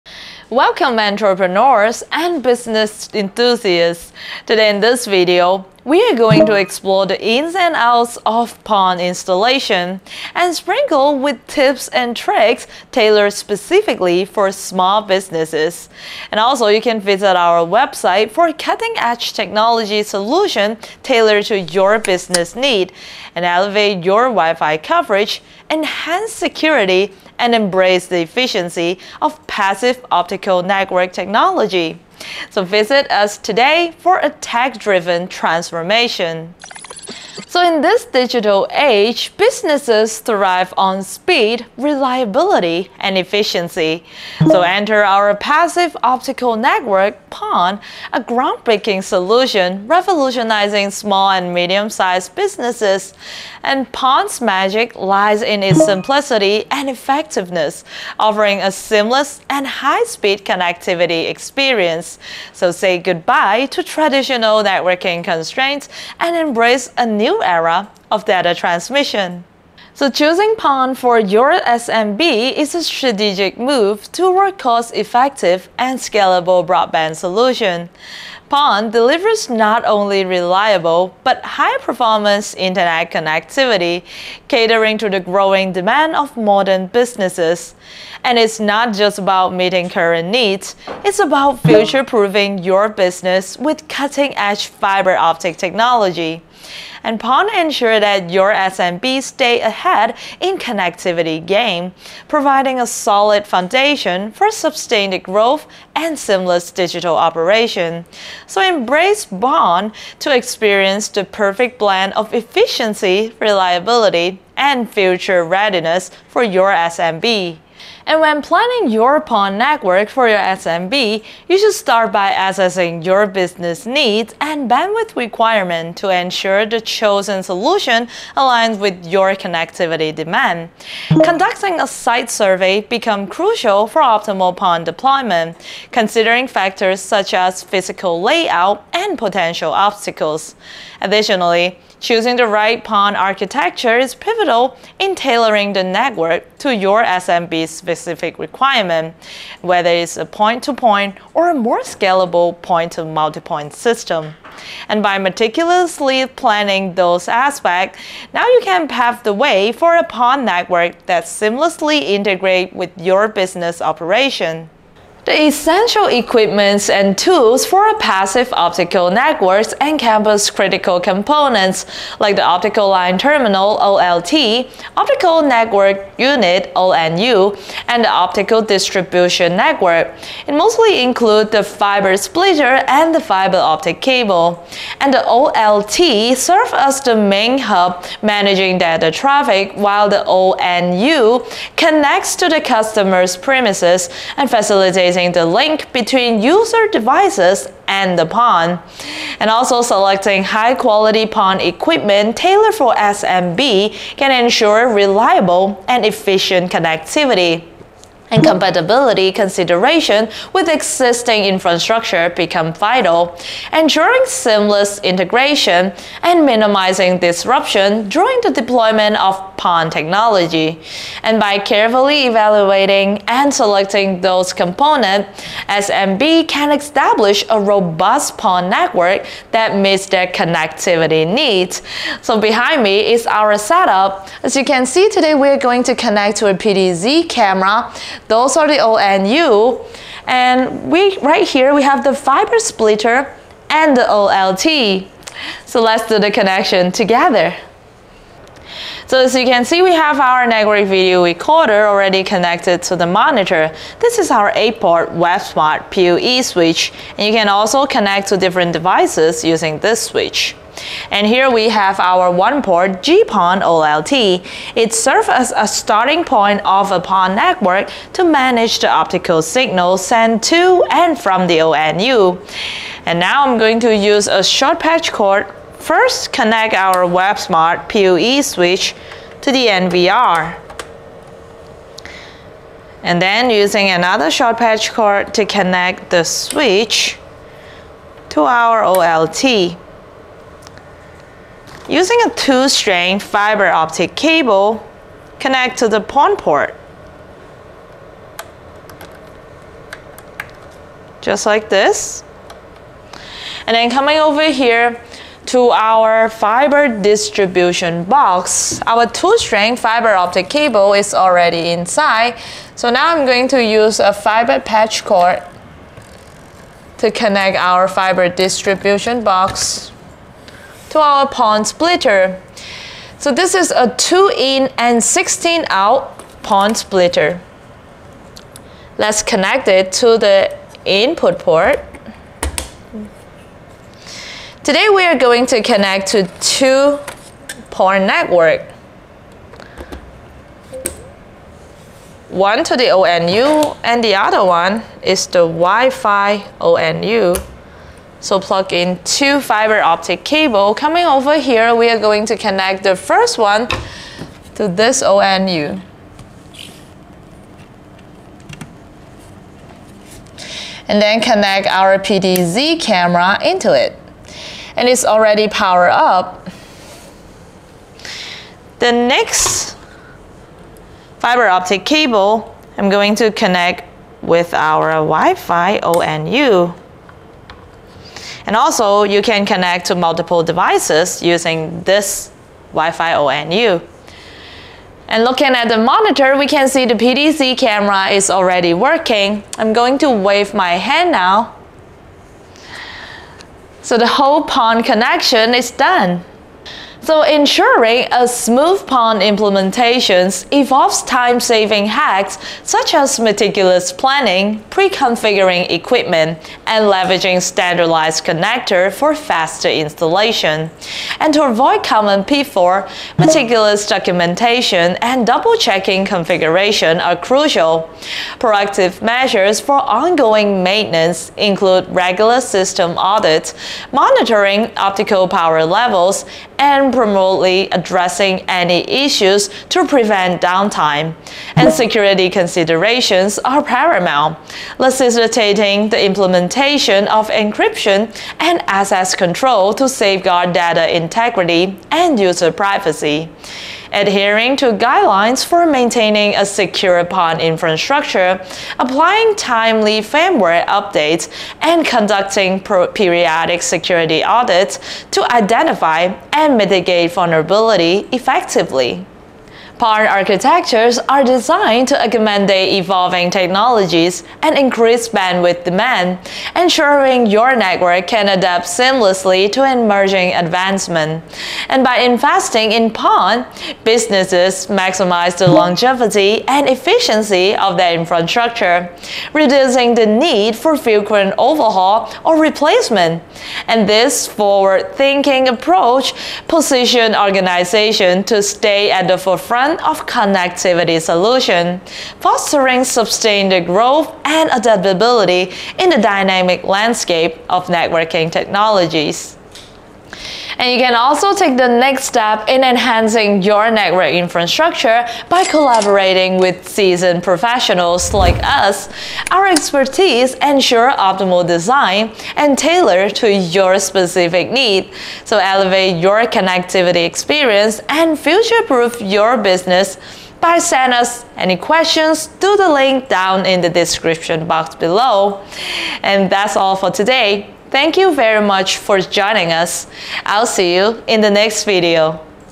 PON Installation for Small Businesses: Tips and Tricks
Unlock the potential of high-speed, reliable connectivity for your small business with our comprehensive guide to PON (Passive Optical Network) installation. In this video, we delve into the world of PON technology, offering valuable insights and tips for a seamless implementation process.

0:00:00 Introduction
Why PON for SMBs?
Planning Your PON Network
Essential Equipment and Tools Overview
Installation Process
Tips and Tricks for a Smooth PON Implementation
Security Considerations
Future-Proofing Your Connectivity